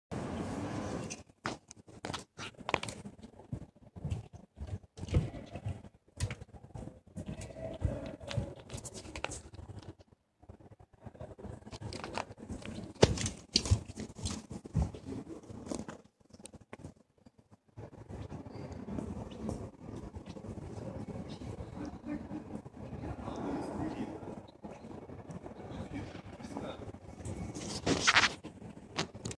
Urban Transport 2017 МТБ-82Д 1777
Троллейбус был построен 1954

ОПИСАНИЕ

Поступил 1 Советский Троллейбусный парк
Построен на заводе им. Урицкого.
Передан МНГПТ
Восстановлен на МТРЗ в ноябре 1993 г.